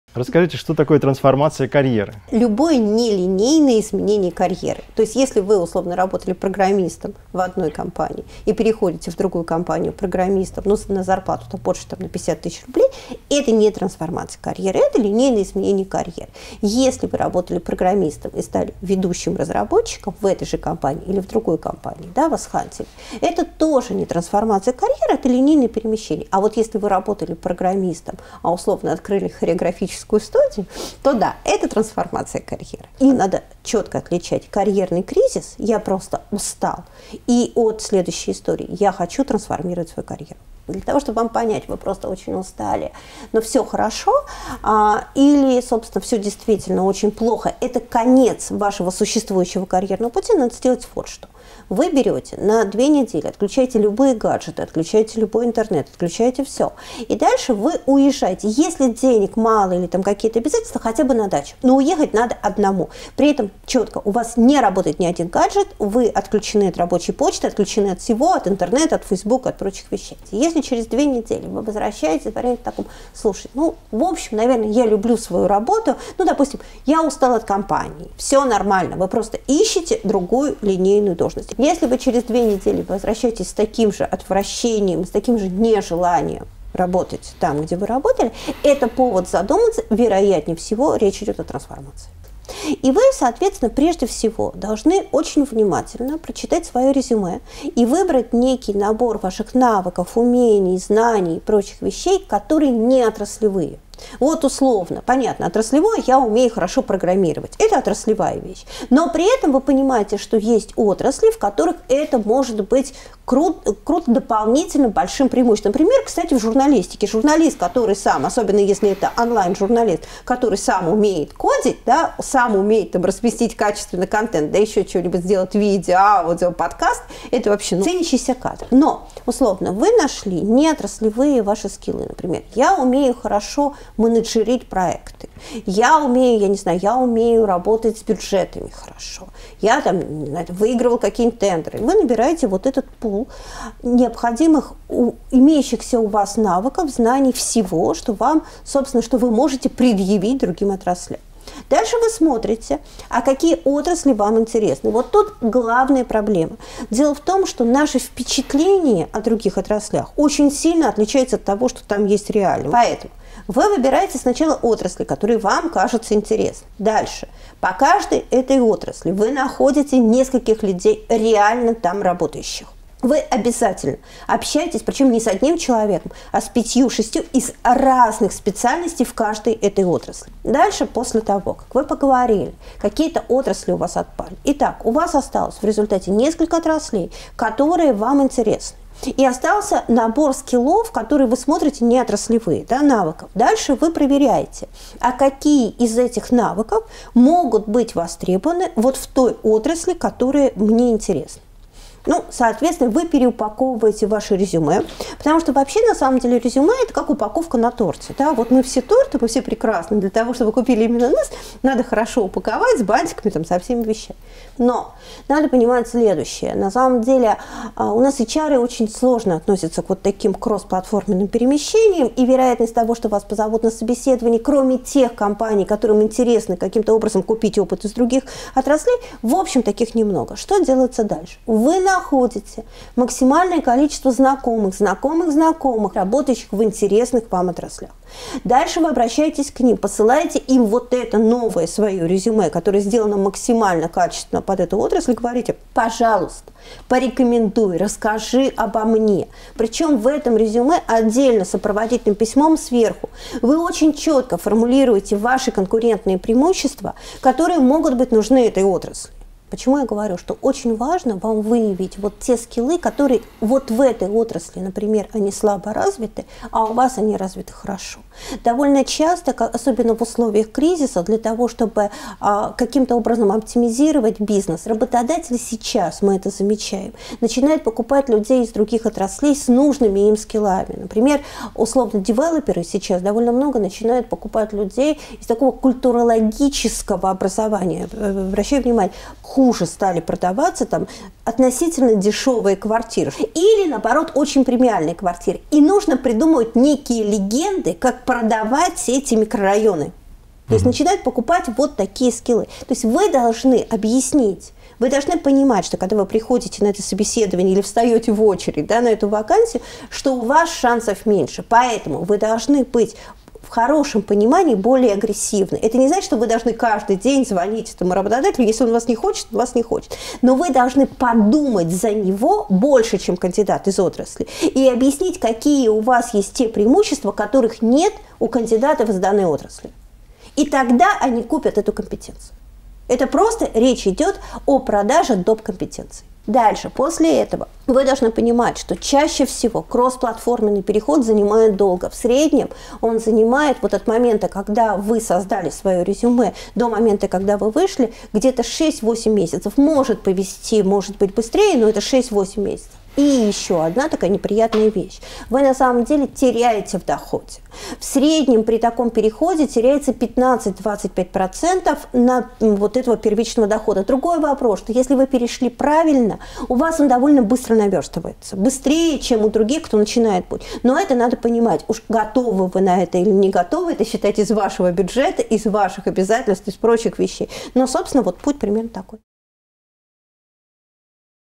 Был программистом, стал артистом балета
Трансформация карьеры. Практические антикризисные советы Алены Владимирской, руководителя агентства Pruffi